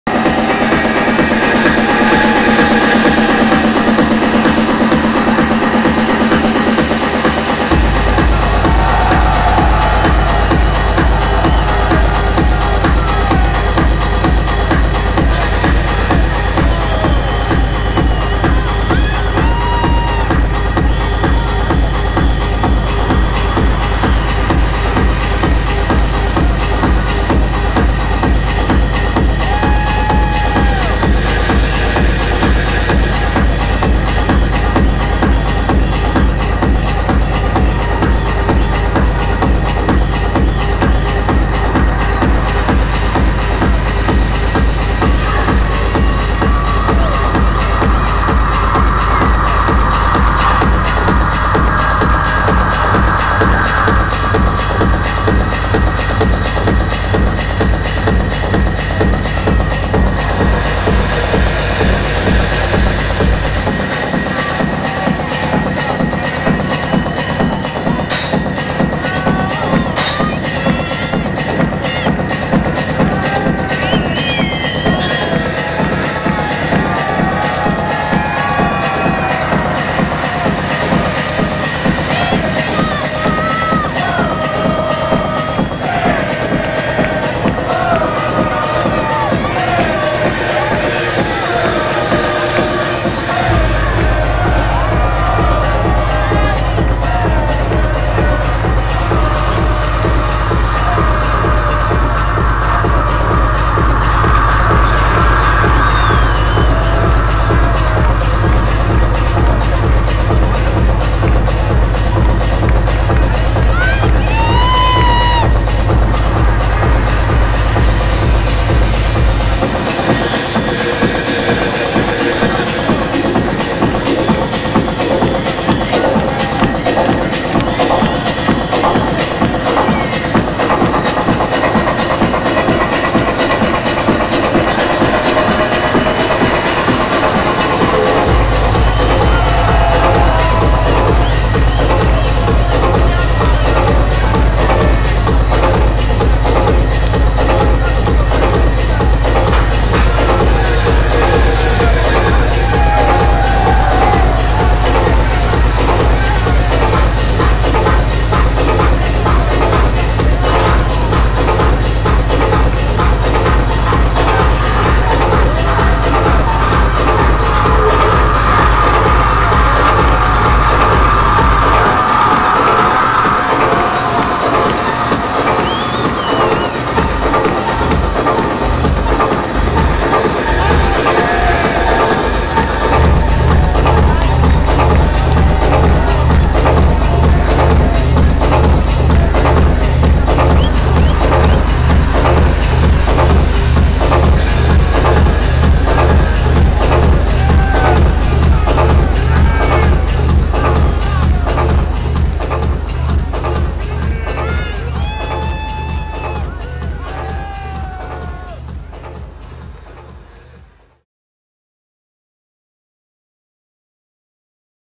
Chris Liebing - Time Warp Italy 2012
Chris Liebing plays some serious techno at Time Warp Italy 2012